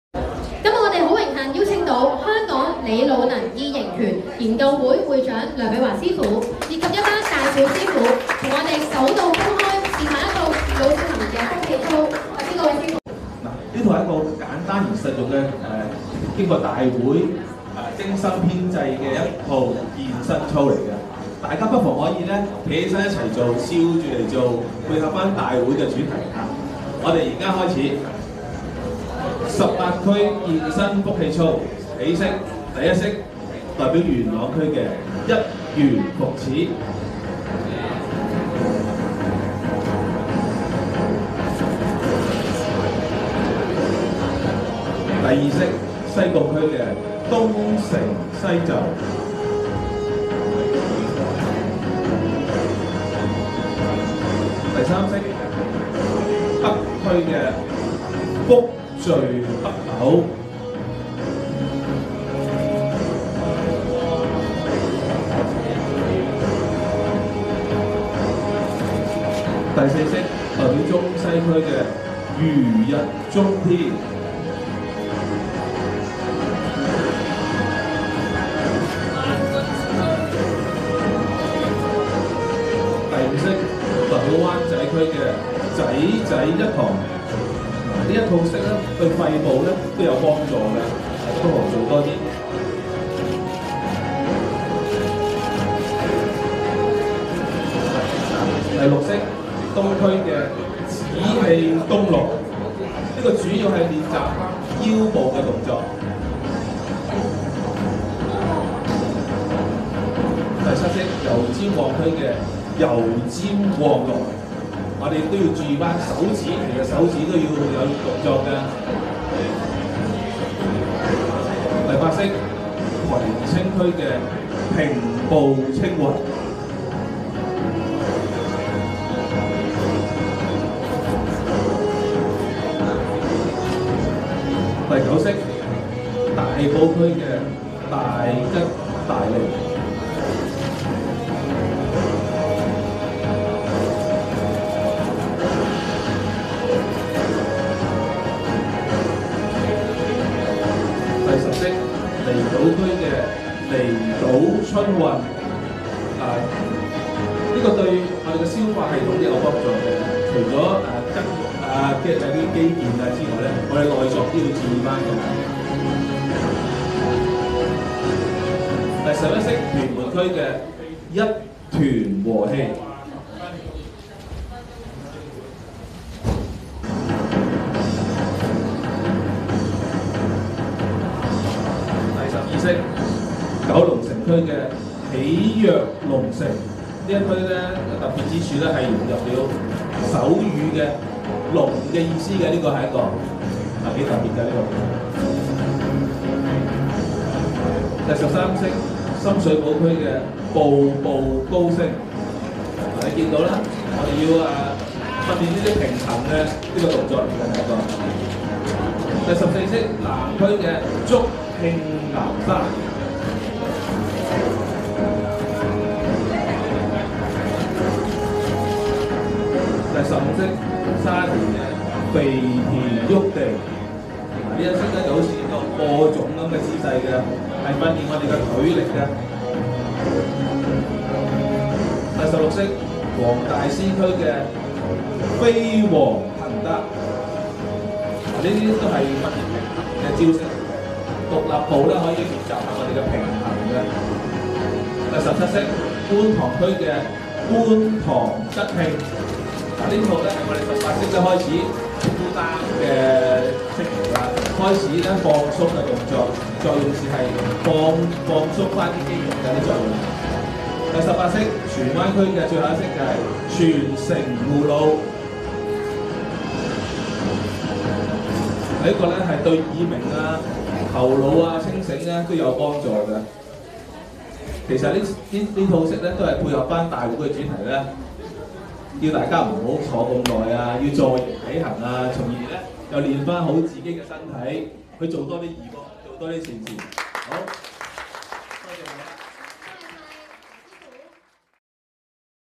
18區福氣操
「傑出生命計劃」以『十八區』的揮春作口訣，首創香港獨有『18區福氣操』於「香港義行日」在全港推行。大會更印刷精裝『福氣操』秘笈，並於全港18區，區區設有福氣站，免費派發，『區區義事、點少得您』。新年將至，義工將打扮成財神到18社區義教『福氣操』、免費派秘笈，以及派福袋給長者，期望人人做完笑呵呵、快樂又健康，全城添福氣。

誠邀各位出席參與今個星期日舉行的第四屆『1.18香港義行日』，今年的「終身成就長者義工大獎」各得獎者將會再次成為大眾的榜樣。活動更獲得政府多個委員會支持，更被納入香港特別行政區成立二十周年慶典項目內。星期日見!